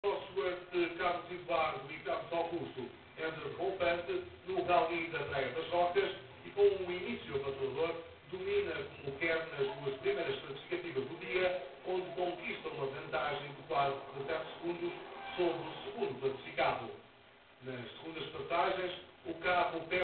Escort Cosworth Carlos Eduardo Team C.A.R.T. Rally Praia das Rocas 2003 Reportagem TV
Carlos Eduardo / Carlos Augusto Rally Praia das Rocas 2003 Reportagem TV